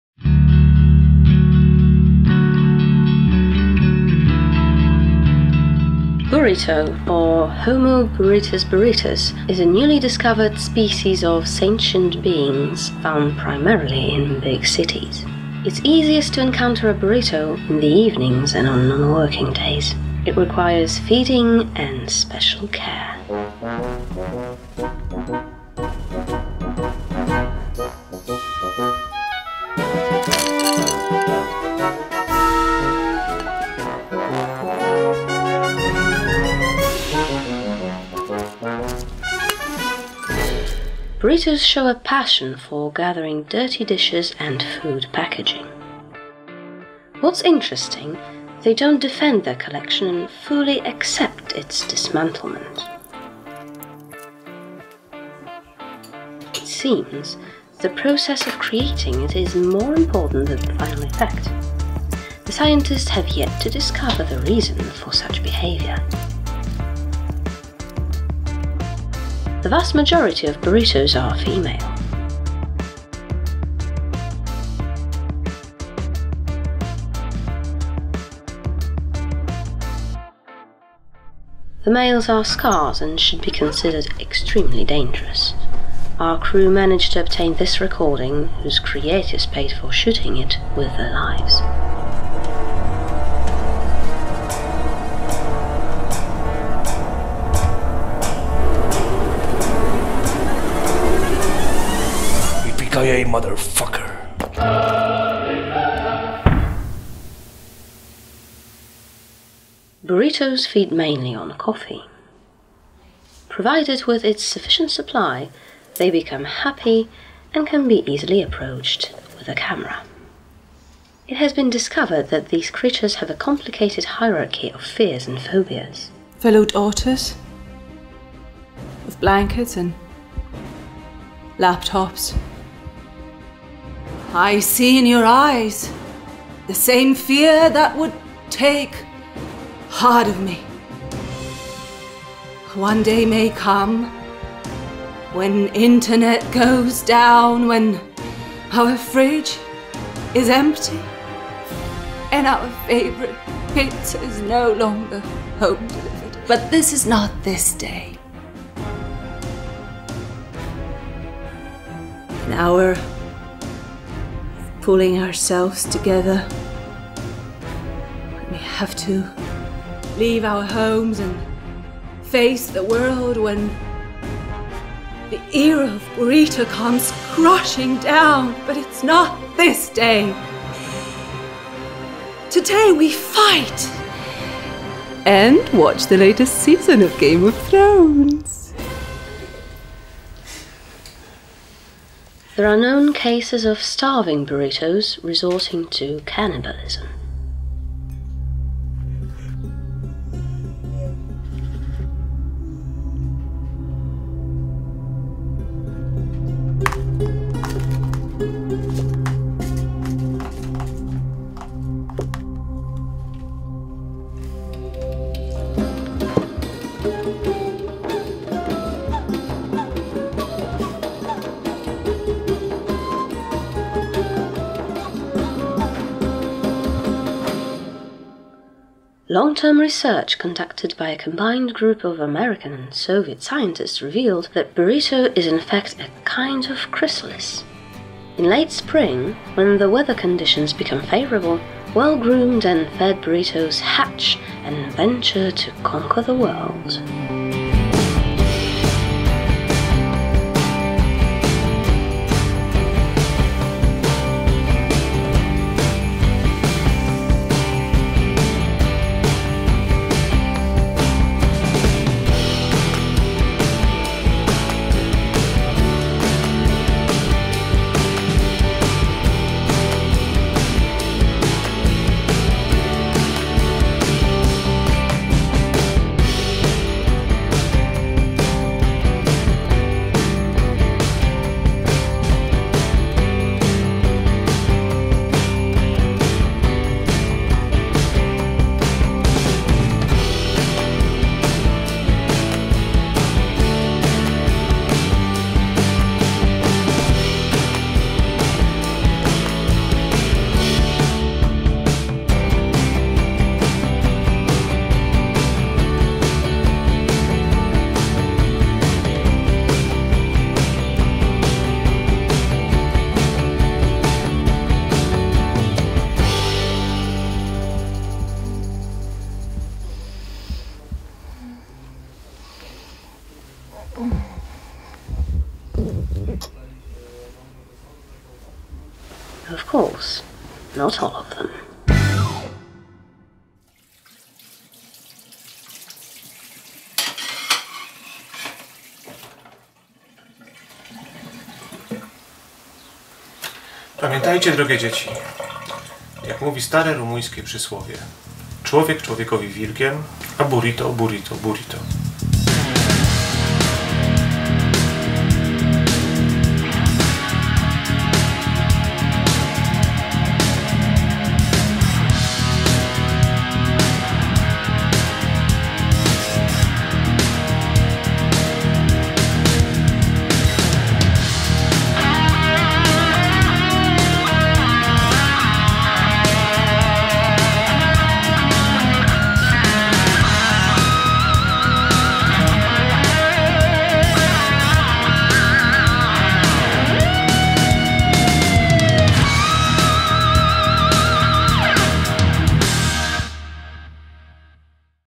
The Burrito ~ Lady Sariel
Such a burrito day
and it's mine~

Film dokumentalny o burrito. Od czasu do czasu, szczególnie przy takiej pogodzie, jak dzisiaj moje życie wygląda dokładnie tak, jak na filmiku :v

Lady Sariel, Jane Bond, Lady Elbereth, Pati, Fallka, Lupus.
Radomir Rytych

Piszcie co sądzicie w komentarzach!